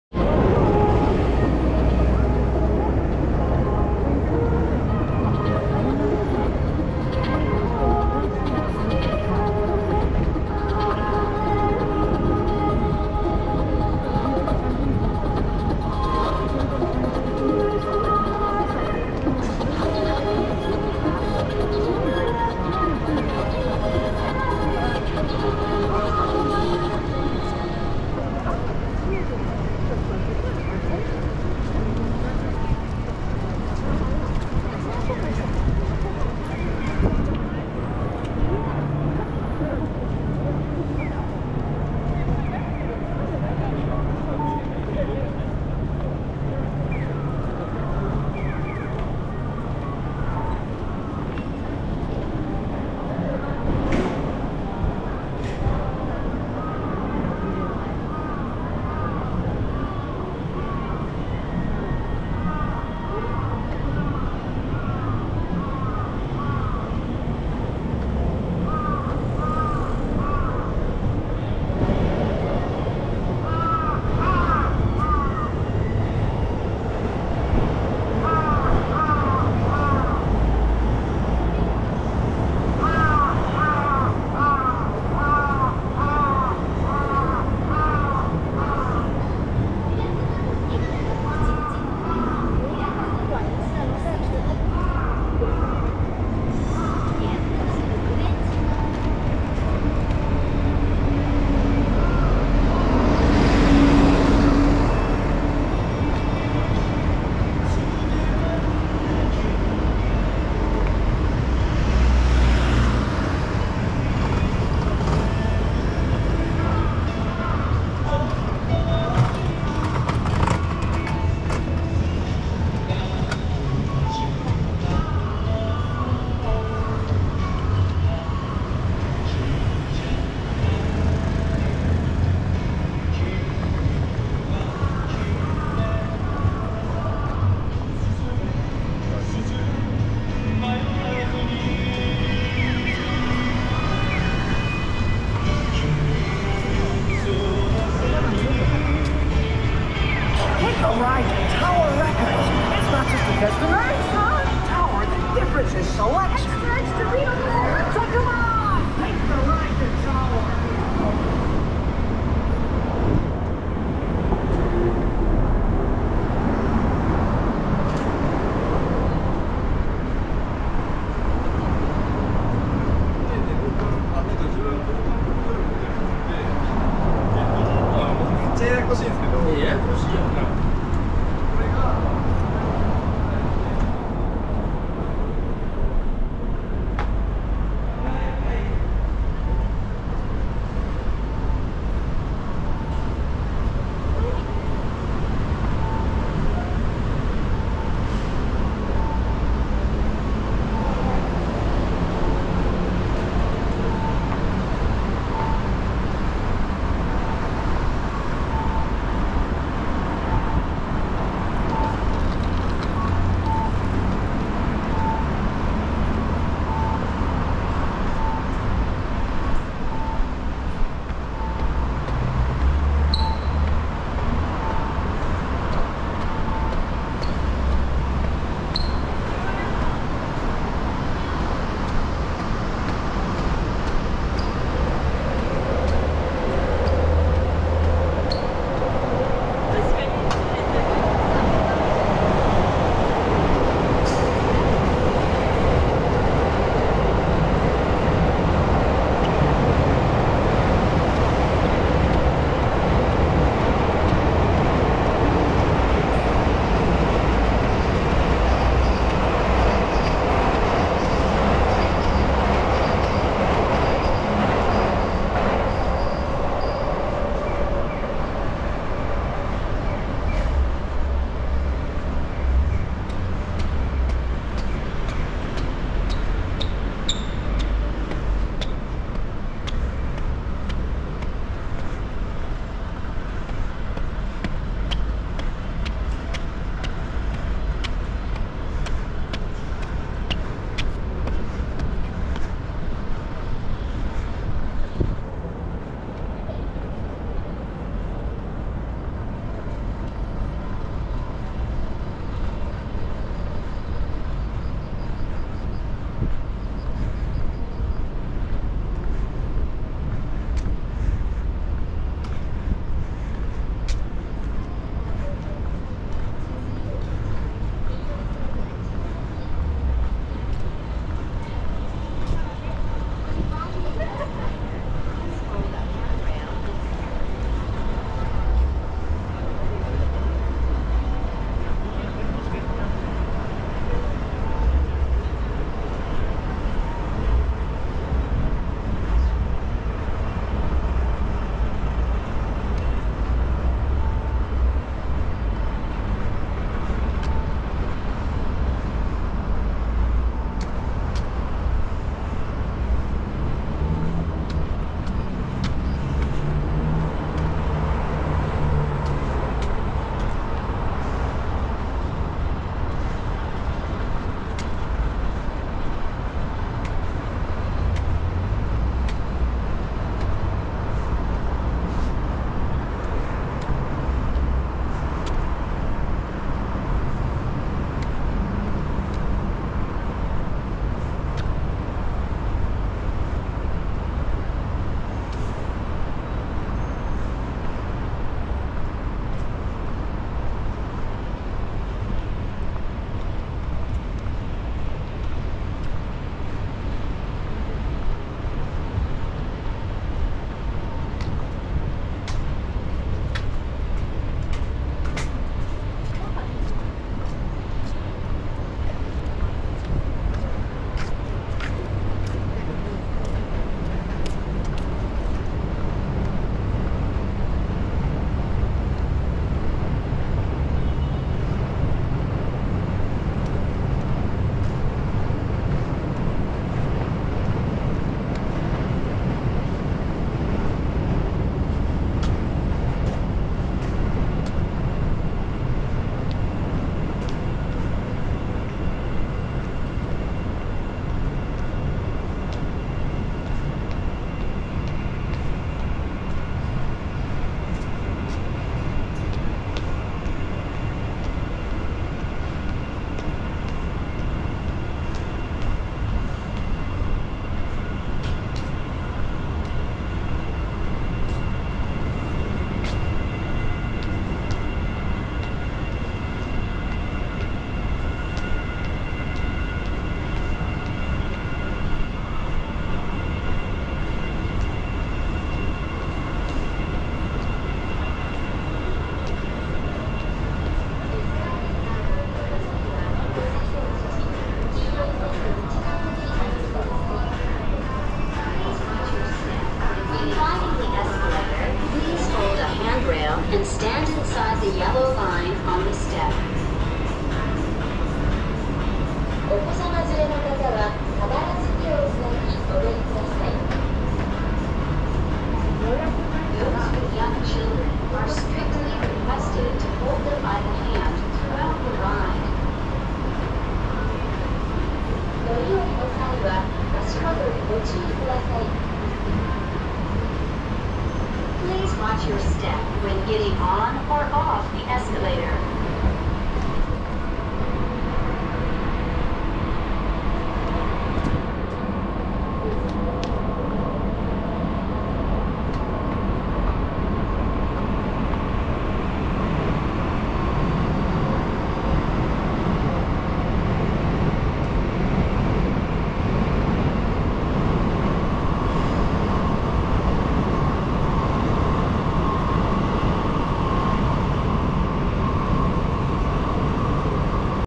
Miyashita Park in Shibuya.🚶🇯🇵 4K [Tokyo]
I am taking a leisurely stroll in Japan.
I want you to experience the excitement of looking at various townscapes and actually wanting to go there.

My Gear
Camera: GoPro hero 10
Camera: iPhone14 Pro Max